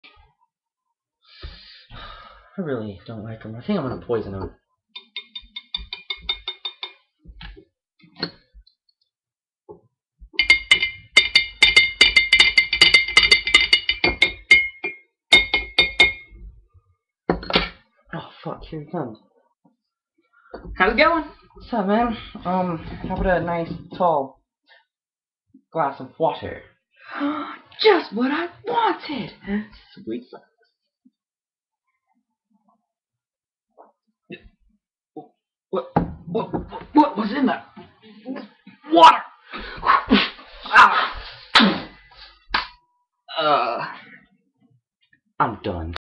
POISON
BILLY JOE GETS POISONED